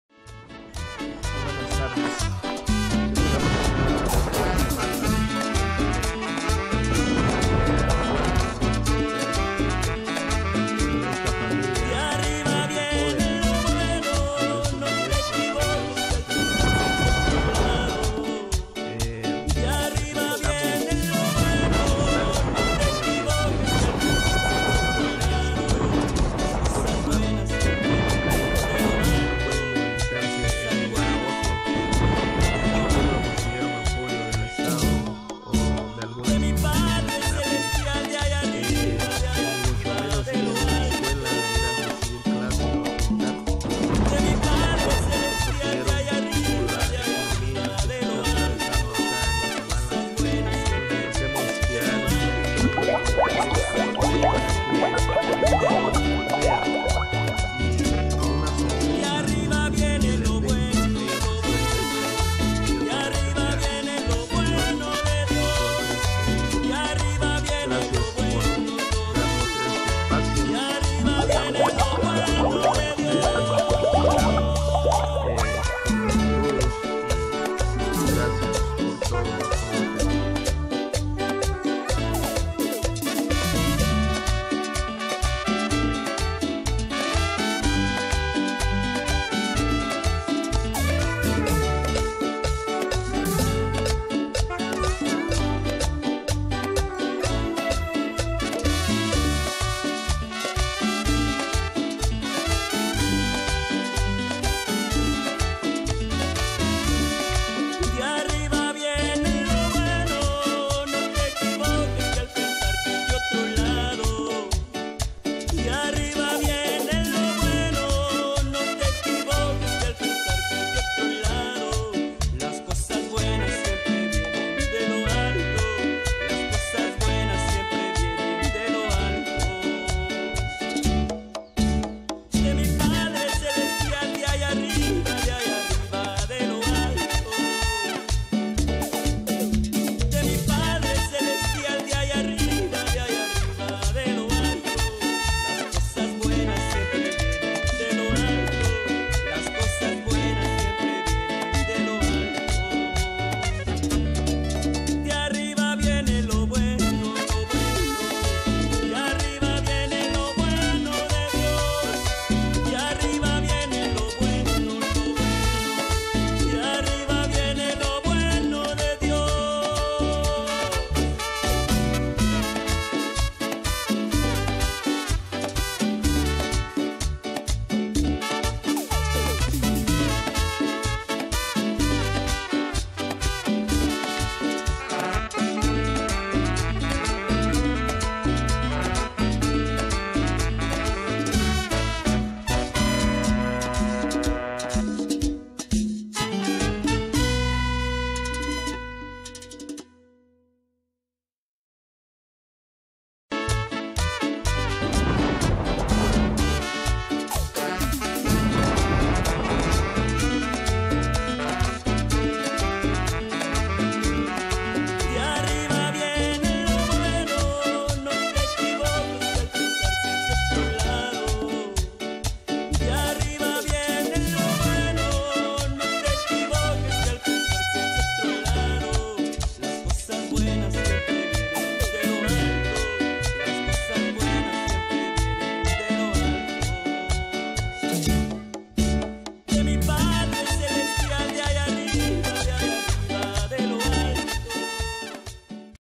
TitleMynor Guanche y Griselda Guanche niños especiales
Viven en aldea vista hermosa san pedro sacatepequez originarios de la aldea panacal rabinal baja vetapaz guatemalaCreated by Video Maker